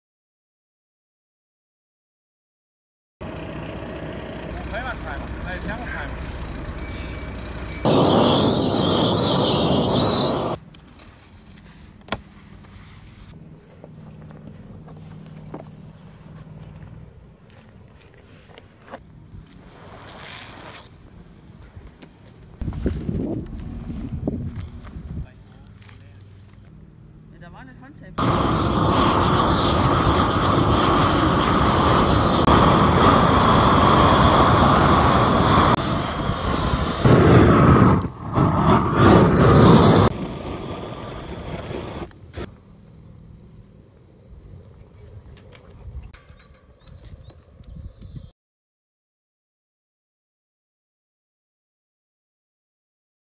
Application of PREMARK® road sign
Preformed thermoplastic road markings can easily be applied with a gas burner and a broom. First clean and prepare the surface, then layout the PREMARK road markings, and then applied the preformed thermoplastic with gas burner. Easy application of road markings, decorative markings and horisontal markings.